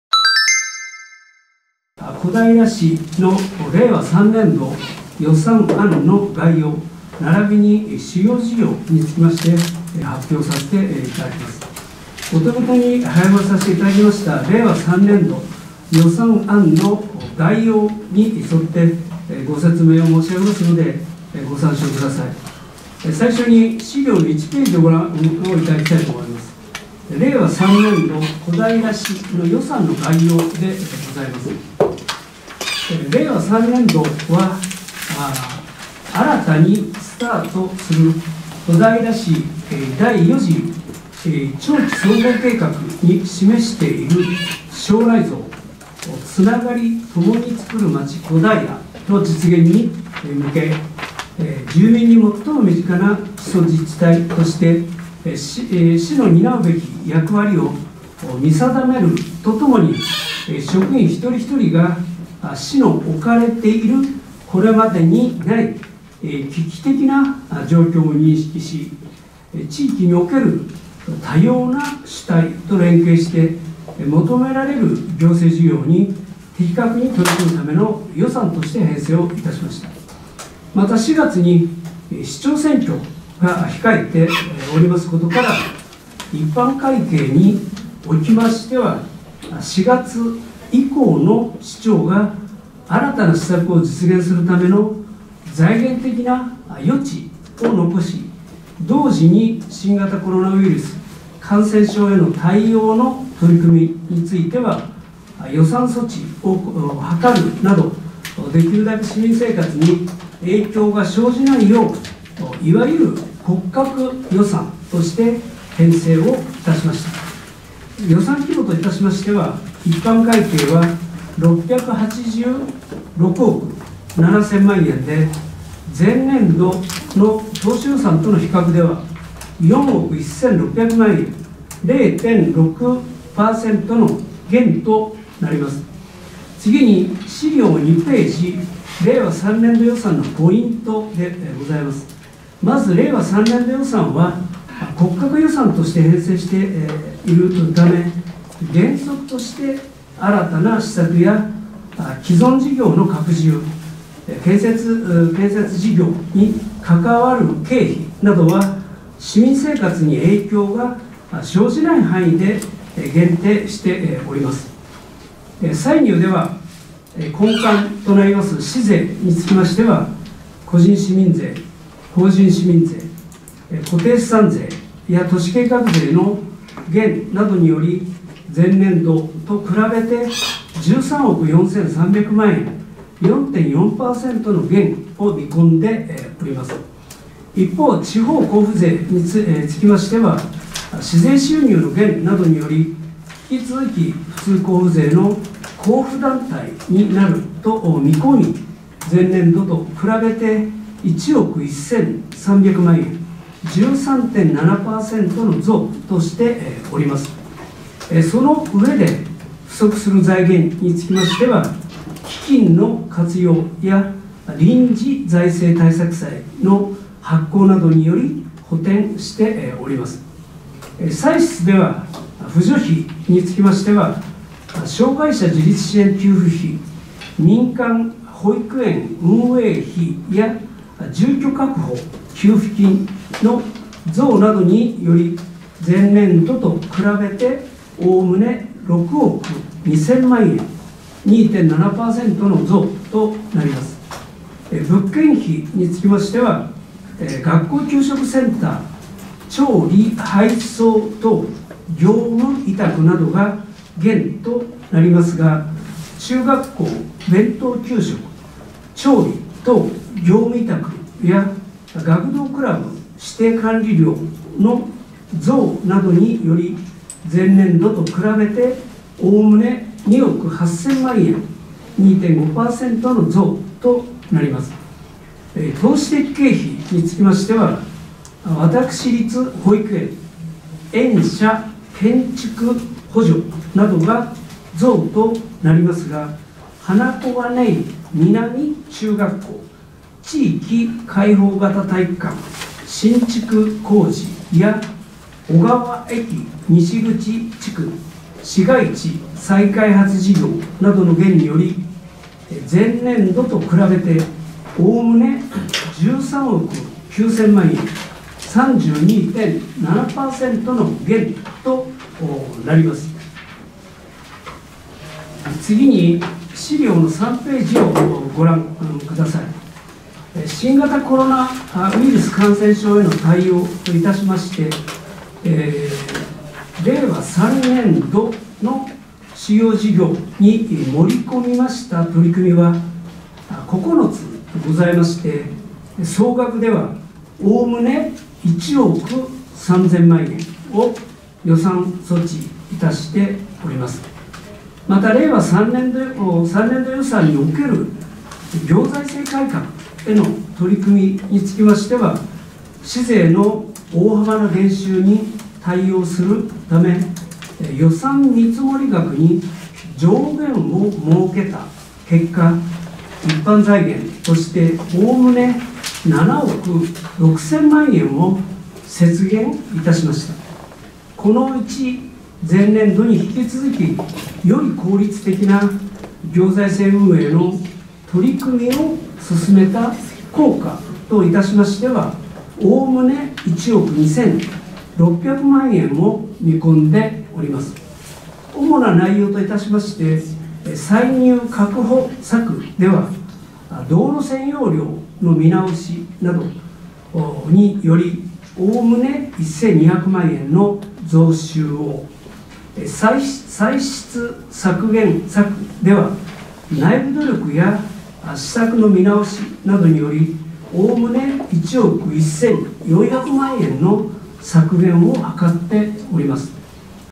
小平市 2021年度の予算案記者会見 ダイジェスト
小平市が2021年度の予算案を記者発表しました。
冒頭の市長からの予算案公表の部分を抜粋してお届けします。

（記事の詳細は「タウン通信」ウェブサイトをご覧ください／